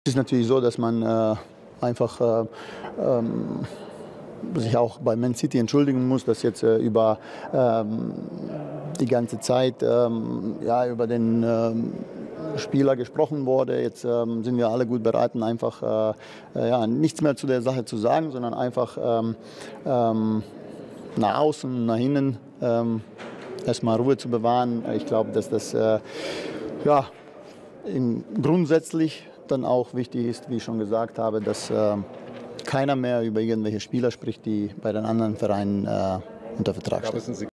El Bayern se disculpa con el City por Sané | Diario As
El director deportivo del Bayern habló ante los medios para pedir disculpas al Manchester City por haber 'tocado' a Leroy Sané, jugador que todavía pertenece al equipo de Guardiola.